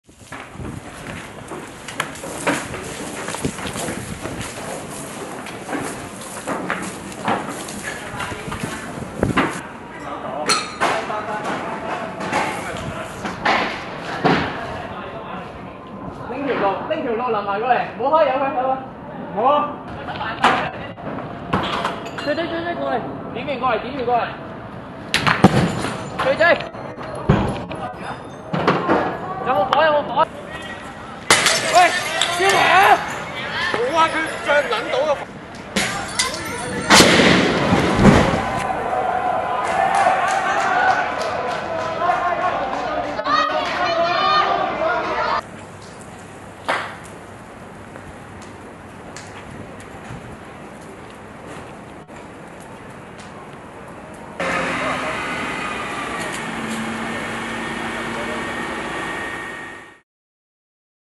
Hong Kong: des manifestants mettent le feu au péage d'un important tunnel | AFP News
Des manifestants pro-démocratie ont mis le feu jeudi soir au péage du Cross-Harbour Tunnel, une voie clé traversant le bras de mer entre l'île et le continent, après une quatrième journée de chaos dans l'ex-colonie britannique.